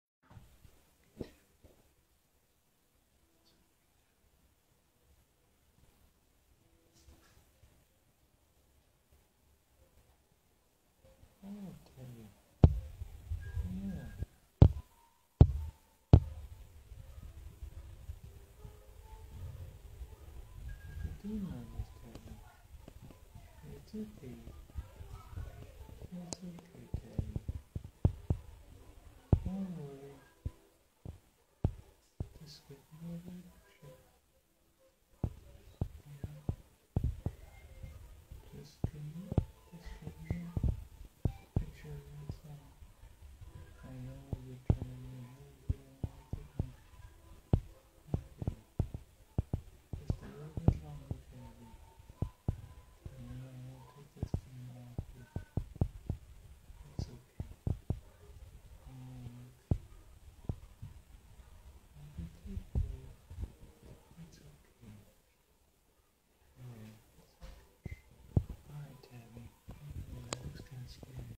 Old film❤️Tabbi 1y Pet Id 815659 shy kitty Humane Society of Southern AZ❤️2-17-2017adopted2-26-2017
❤️Tabbi 1y Pet Id 815659 female shy kitty Humane Society of Southern Arizona❤️3450 N Kelvin Blvd Tucson AZ Phone on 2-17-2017. P.S. I think the grey coat is a cool color of kitty & it shows nicely if the kitty has black stripes.